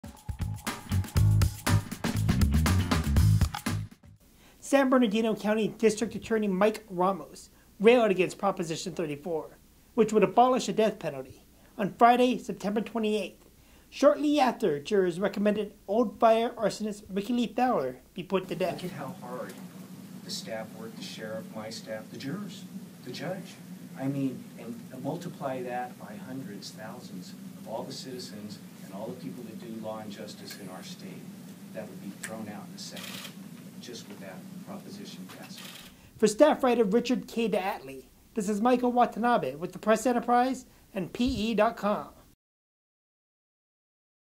PROP. 34: Ramos rails against abolition of the death penalty
San Bernardino County Mike Ramos voiced opposition to Prop. 34 shortly after jurors recommended death for Old Fire arsonist Rickie Lee Fowler.